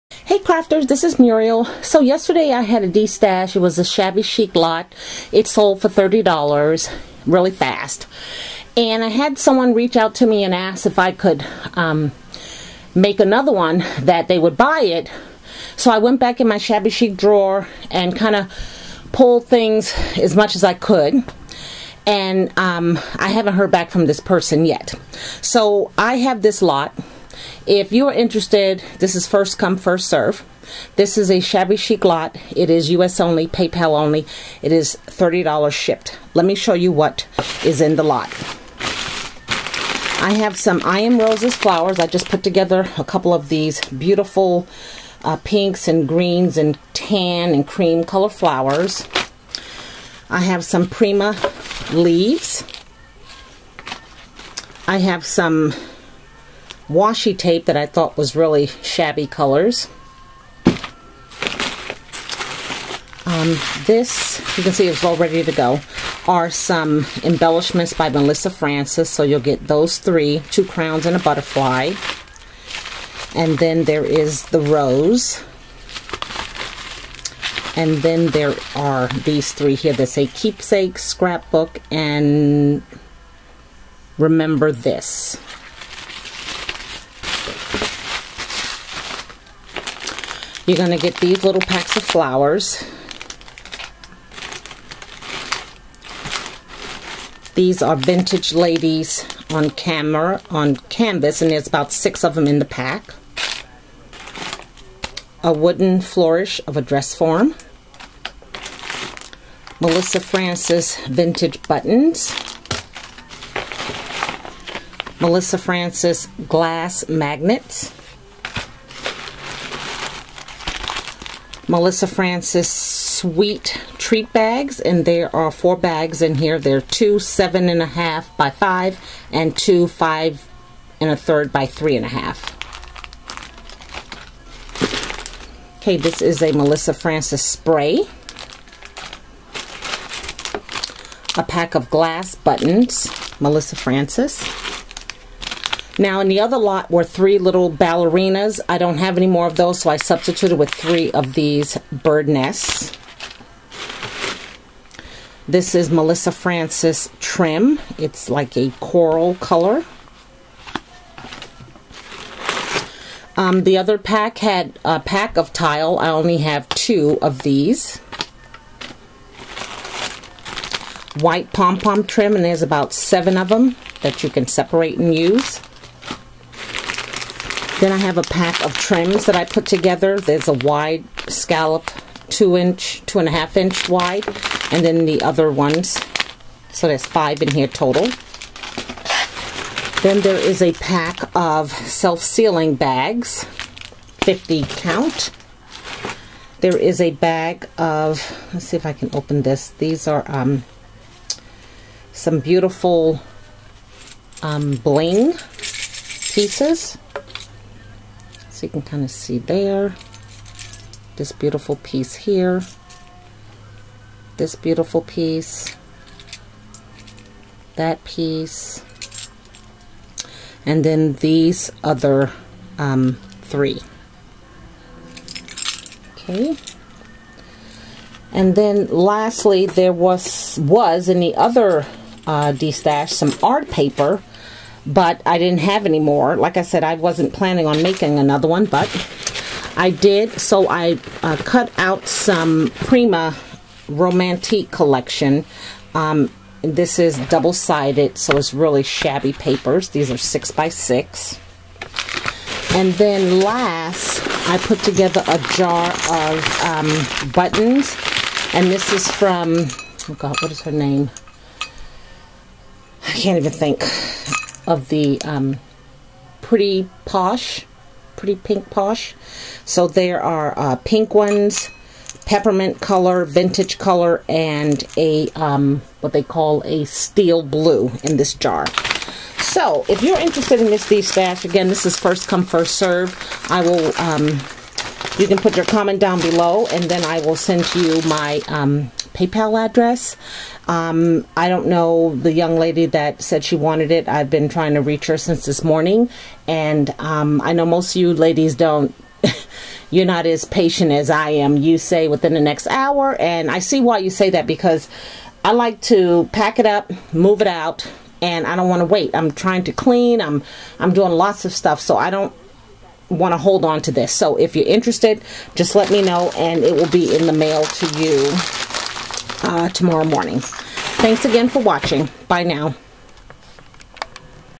Shabby Chic Lot Destash $30.00 first come first serve **(SOLD**)
I will not be holding this lot.  If it's not purchased within the hour, back in the Shabby Vault it goes.